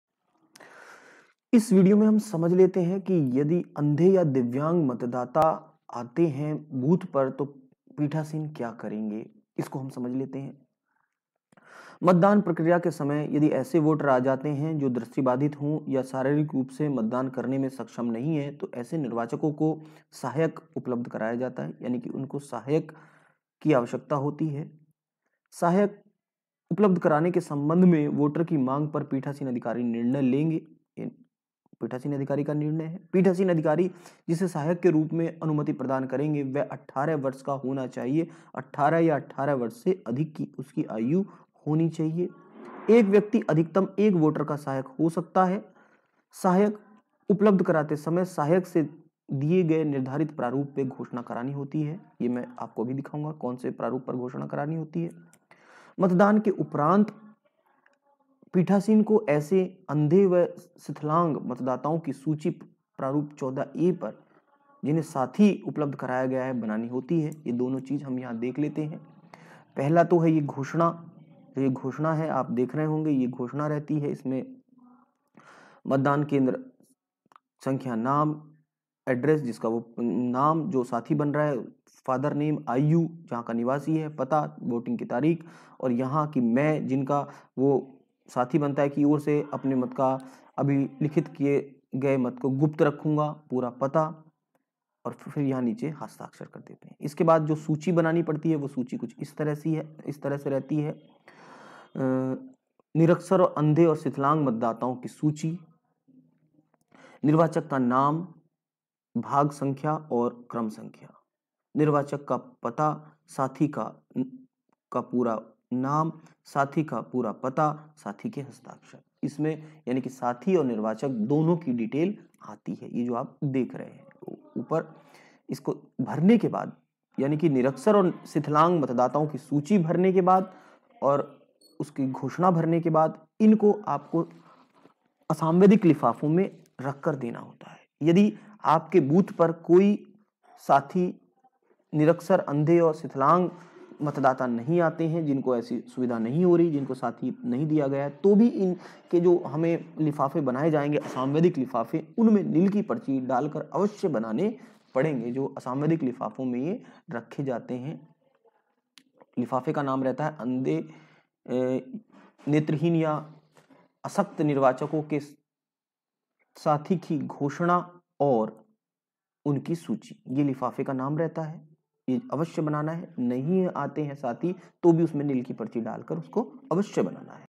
नेत्रहीन / निशक्त मतदाताओं की सूची और सहयोगी के घोषणा होने या ना होने पर पीठासीन को क्या करना होगा
*विशेष परिस्थिति जब वोटर वोट डालने को माना कर दे और उस की एंट्री मतदाता रजिस्टर में हो जाये, तो पीठासीन को क्या करना होगा*